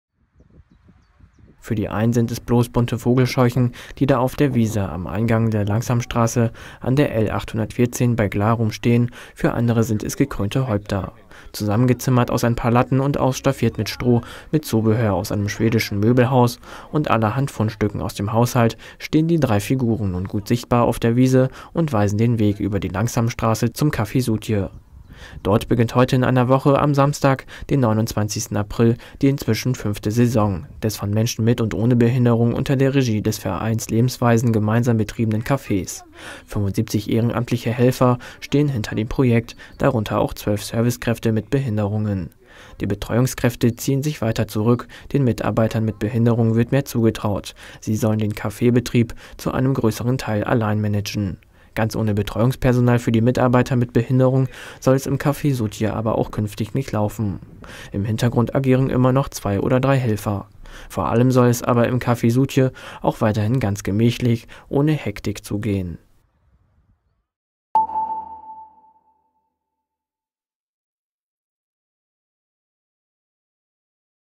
Café Suutje öffnet in einer Woche
Schortens, 21. April 2017: Am Samstag, den 29. April, beginnt die inzwischen fünfte Saison des von Menschen mit und ohne Behinderung unter der Regie des Vereins „Lebensweisen“ gemeinsam betriebenen Cafés. 75 ehrenamtliche Helfer stehen hinter dem Projekt, darunter auch zwölf Servicekräfte mit Behinderungen.